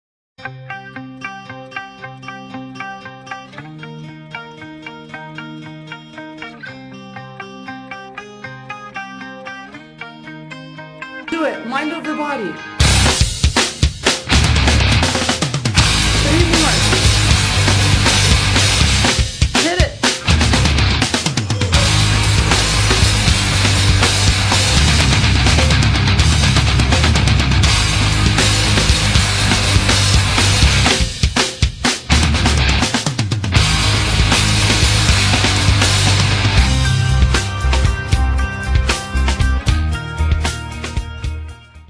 Mind Over Body: Handstand push ups (Firsthand Fitness)
Mind Over Body Demo by Raji Zreik , Handstand push-ups, for blog entry
I always tell my clients! Mind over body! you can do it now! come on, push it to the next level.